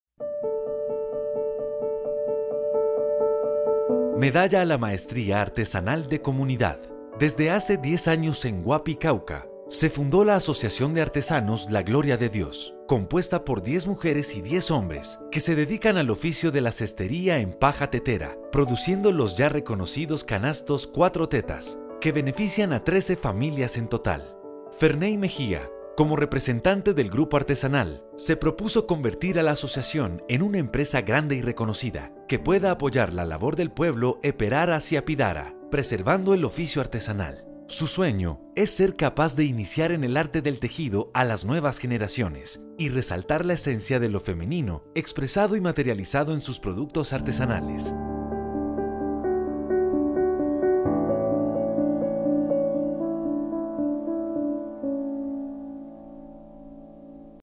Medalla a la Maestría Artesanal de Comunidad, 2019
Desde hace diez años en Guapi, Cauca, se fundó la Asociación de Artesanos La Gloria de Dios; compuesta por artesanas y artesanos que se dedican a la cestería en "paja tetera", con la cual elaboran los ya reconocidos canastos  "cuatro tetas".